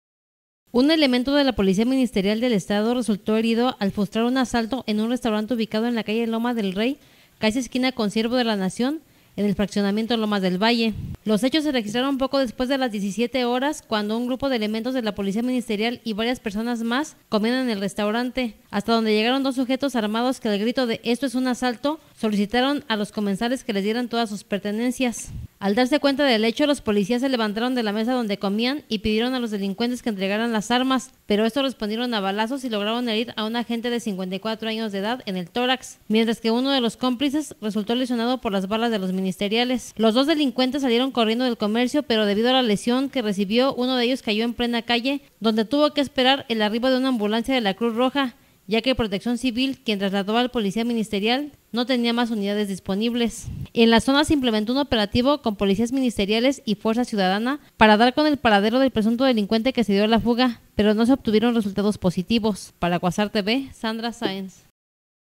Ministerial resulta lesionado al frustrar asalto en Lomas del Valle
Un elemento de la policía ministerial del estado resultó herido al frustrar un asalto en un restaurante ubicado en la calle Loma del Rey, casi esquina con Siervo de la Nación, en el fraccionamiento Lomas del Valle…